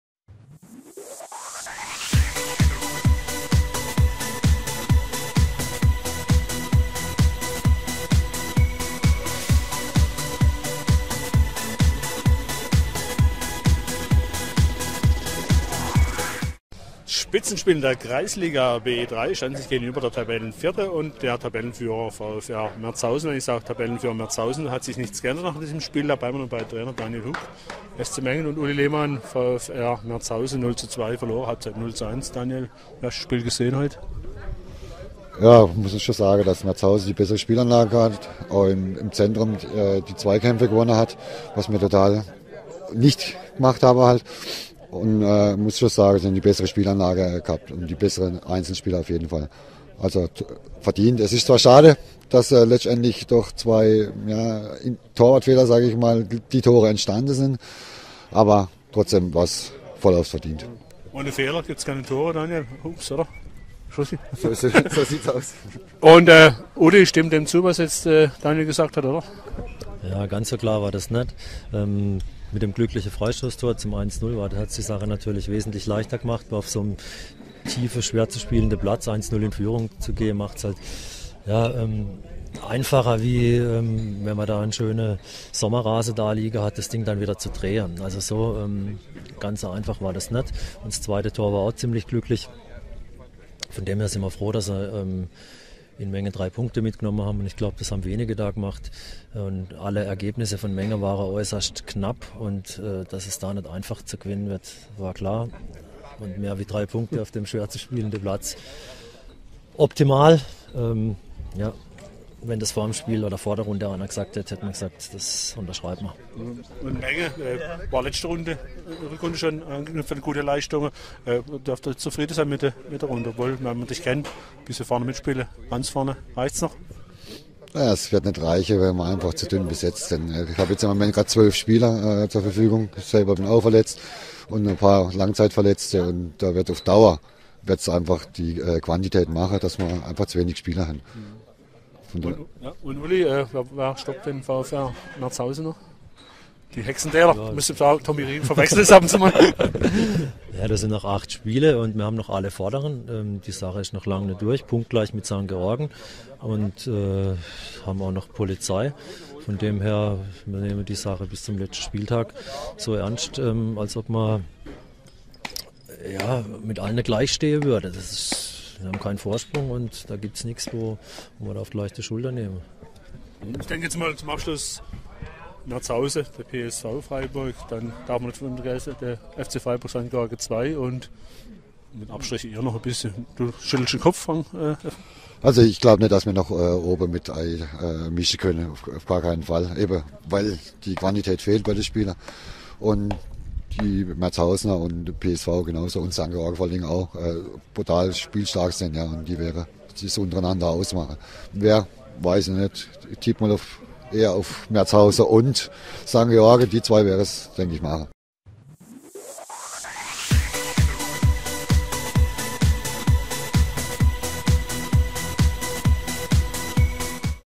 DP-WEB-RADIO: Spitzenspiel SC Mengen - VfR Merzhausen 0:2
Kreisliga B3: Die beiden Trainer Uli Lehmann (VfR Merzhausen) und Daniel Hug (SC Mengen),beides früher langjährige Verbandsligaspieler!